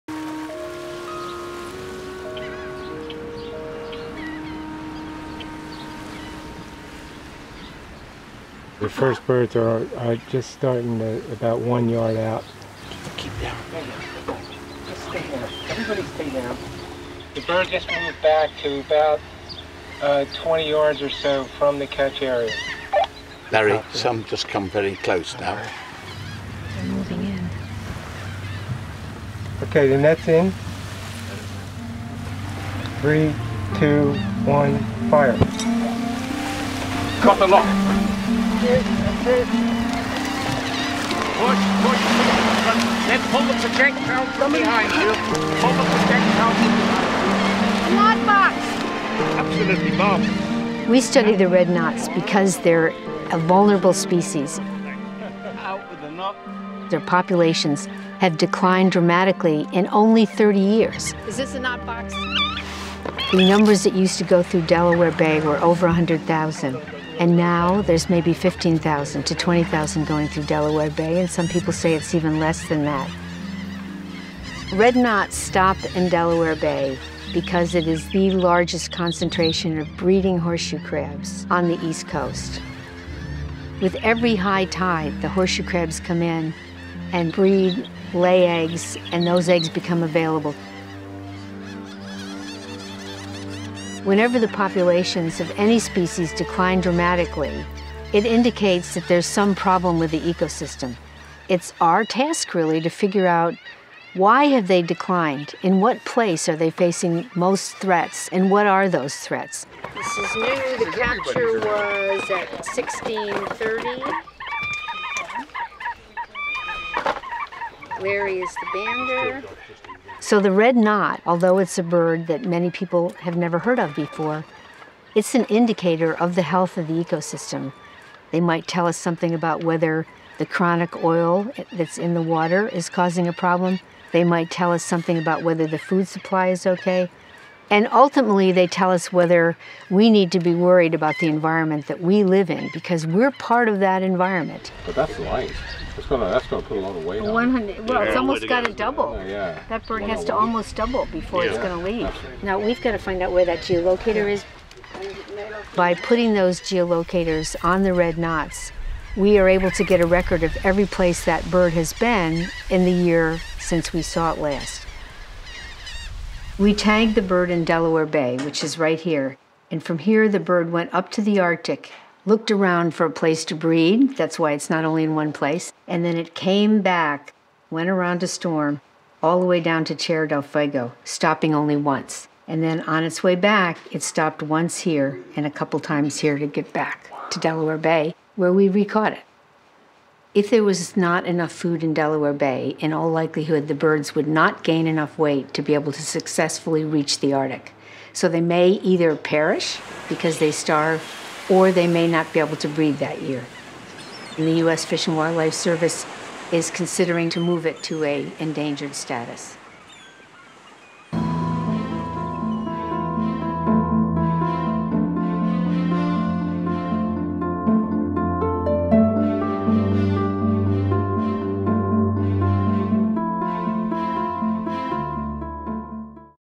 Red Knots and Horseshoe Crabs of Delaware Bay
Marine Defender Dr. Joanna Burger, a professor at Rutgers University's Department of Ecology, Evolution, and Natural Resources, is a specialist in shore birds like the Red Knots, which are remarkable navigator which are one of the Earth's longest distance migrants. The Red Knot was listed as a federally threatened species under the United States Endangered Species Act in the fall of 2014.

Please visit marinedefenders.org for more information about marine oil spills.

Produced by Common Good Productions for the Marine Defenders Project.